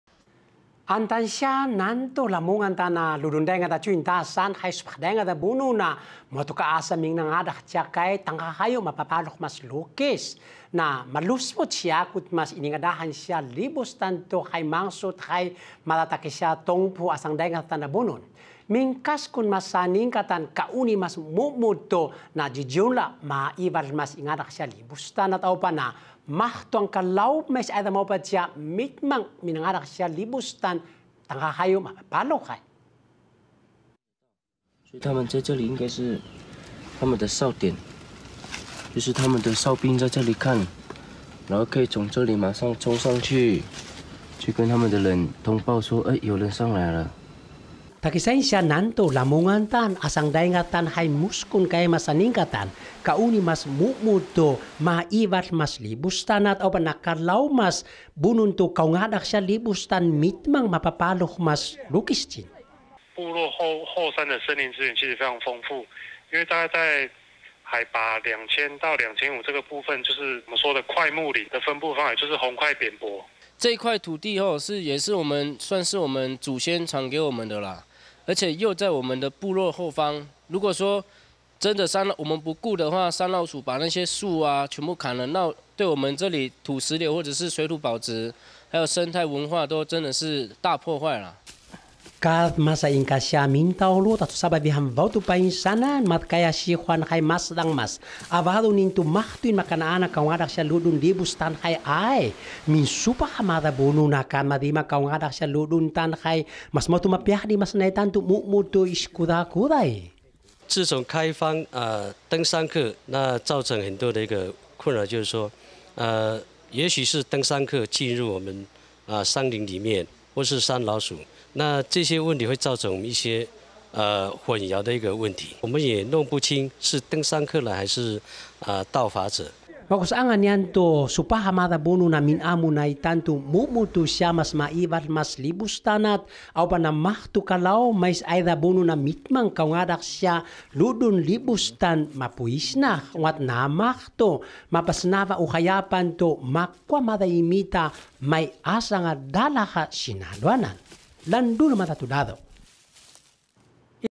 遏止盜伐 東埔村.林務局合作成立巡山護林隊2020-10-29 Bunun IPCF-TITV 原文會 原視族語新聞
Mingkaskun a asang mas Saningkatan ma'ival libus

位在南投信義鄉的郡大山，是許多不法人士覬覦的地區，為了維護當地山林資產，東埔村的族人和林務局合作成立巡護隊，降低盜伐業者侵犯山林。

南投信義東埔村與林務局合作成立巡山護林隊，制止不法人士破壞當地珍貴的檜木原始林。

不過自從108年政府開放山林政策，全台掀起爬山熱潮，讓護林隊增添不少困擾.

希望有更多族人一起參與護林工作，除了更能制止山林環境生態遭到侵犯，也可以教導族人認識自己族群的傳統領域。